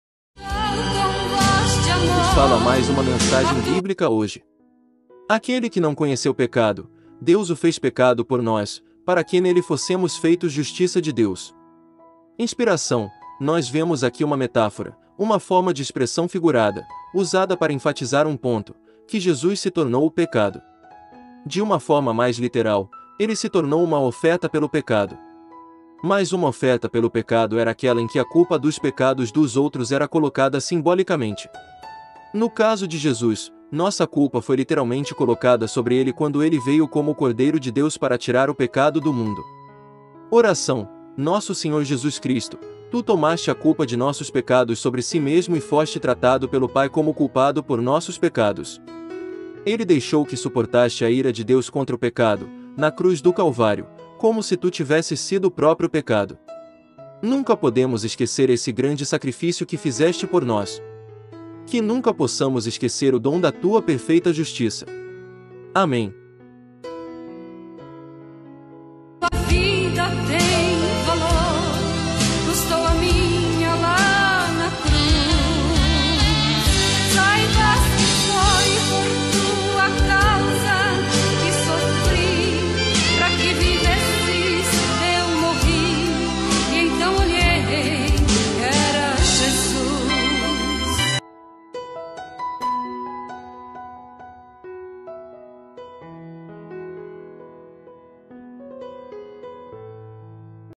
Mensagem Bíblica e Oração 2 Coríntios 5:21
Àquele que não conheceu pecado, Deus o fez pecado por nós; para que nele fôssemos feitos justiça de Deus.
Inspiração: Nós vemos aqui uma metáfora, uma forma de expressão figurada, usada para enfatizar um ponto: que Jesus se tornou "pecado". De uma forma mais literal, ele se tornou uma "oferta pelo pecado". Mas uma oferta pelo pecado era aquela em que a culpa dos pecados dos outros era colocada simbolicamente,. No caso de Jesus, nossa culpa foi literalmente colocada sobre ele quando ele veio como o Cordeiro de Deus para tirar o pecado do mundo.
Oração : Nosso Senhor Jesus Cristo, tu tomaste a culpa de nossos pecados sobre Si mesmo e foste tratado pelo Pai como culpado por nossos pecados. Ele deixou que suportaste a ira de Deus contra o pecado, na cruz do Calvário, como se Tu tivesses sido o próprio pecado. Nunca podemos esquecer esse grande sacrifício que fizeste por nós. Que nunca possamos esquecer o dom da Tua perfeita justiça. Amém.